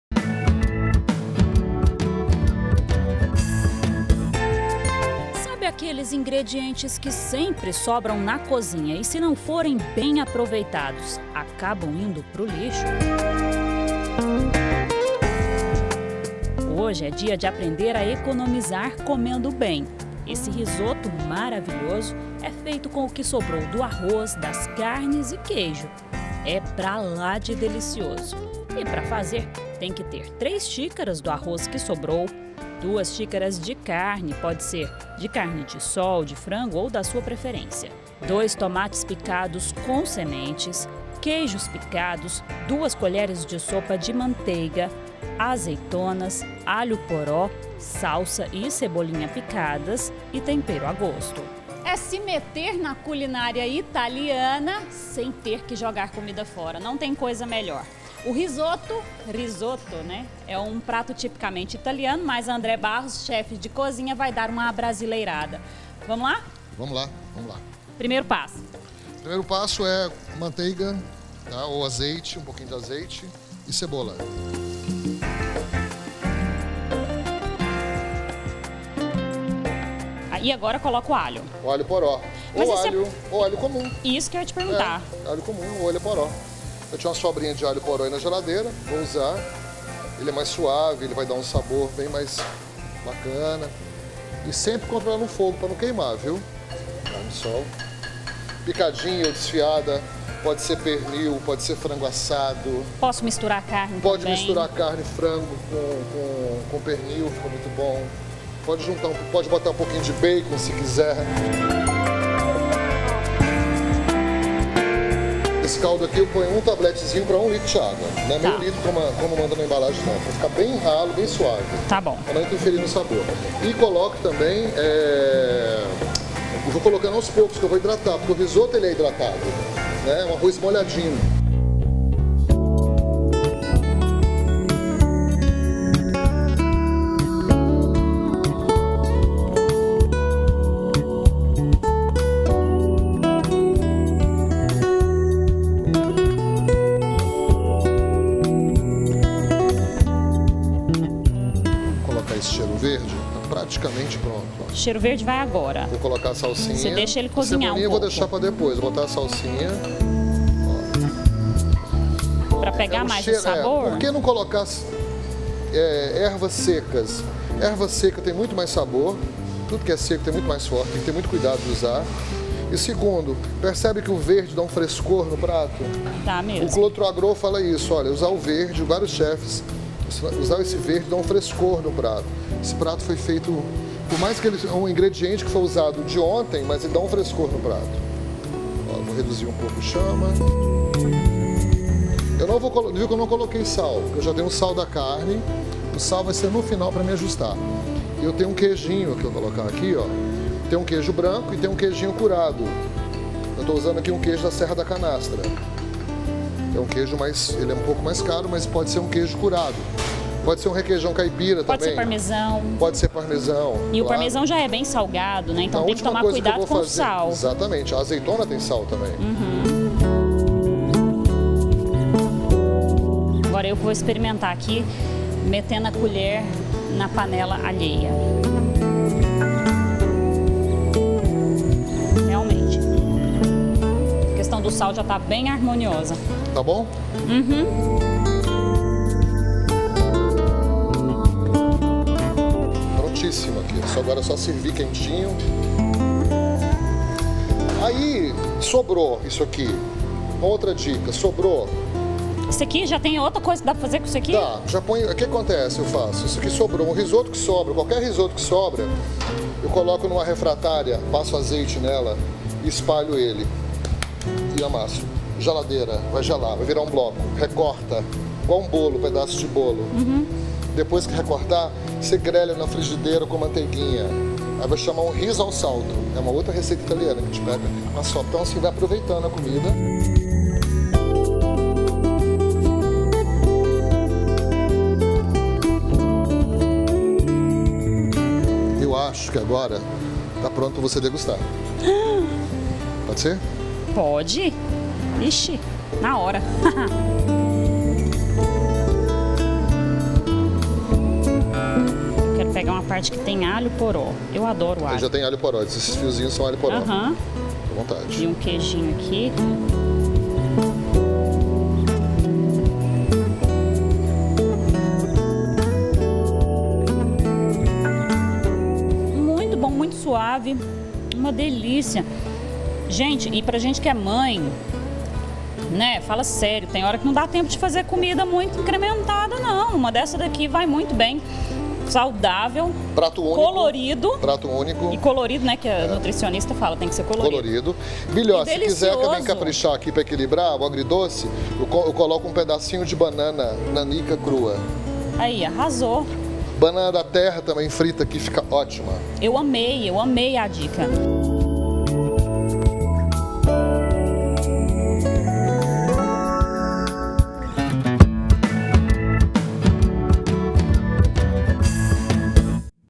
APRENDA A FAZER RISOTTO: RECEITA REAPROVEITA ALIMENTOS
Chegou a hora do quadro prato feito. Você vai aprender a fazer uma receita de risotto aproveitando a sobra de alimentos.